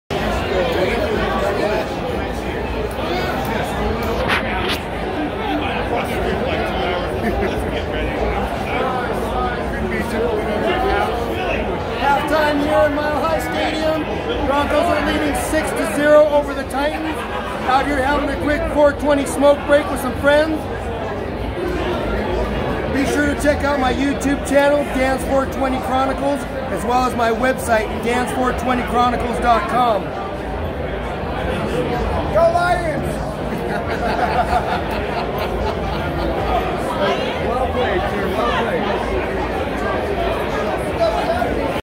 Dan's 420 Chronicles - 420 Football Game Clip 2 - 10/13/19
Dan's 420 Chronicles at the Denver Broncos Game at Mile High Stadium.

Check out my blog, concert reviews, and pictures from concerts as well as other 420 events